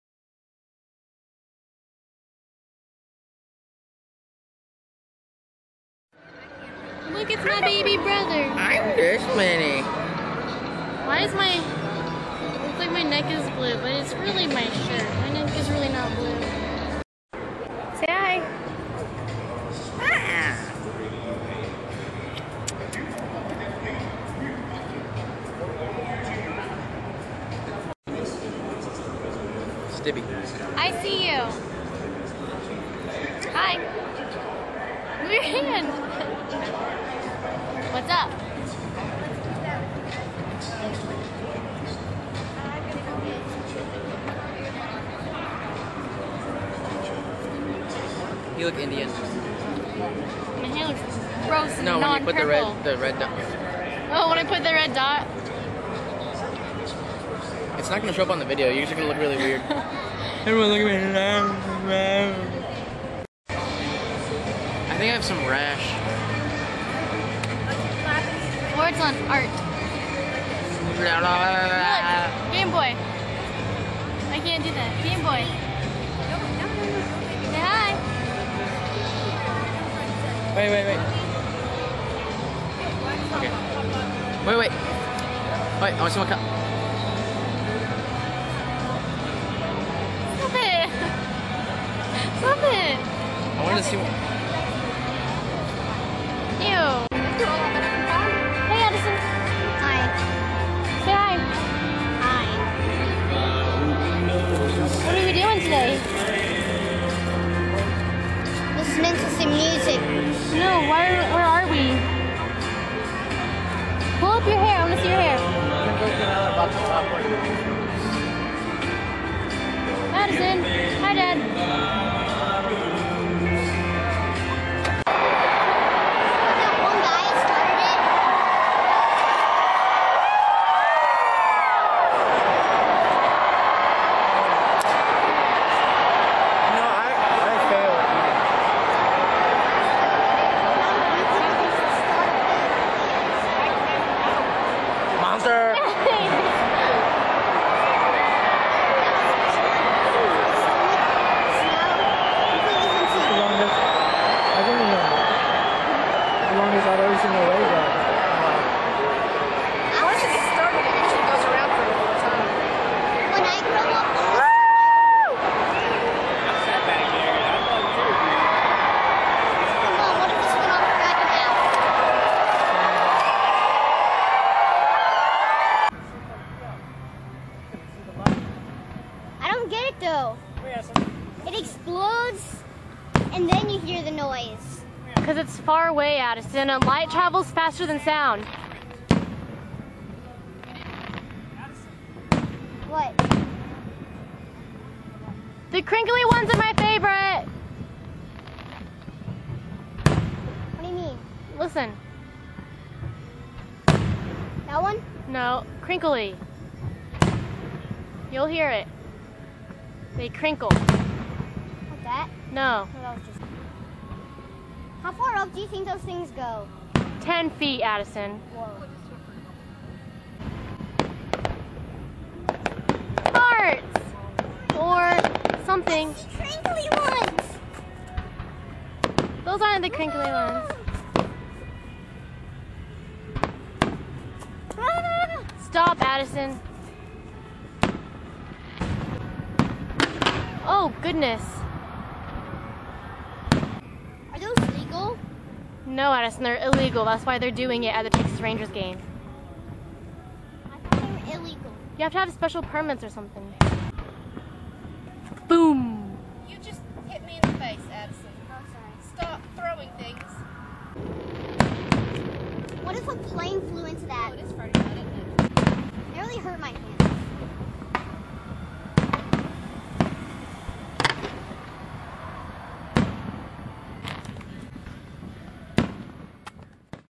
Ranger Gamee
Before, during, and after the Ranger game. Which we won by the way :)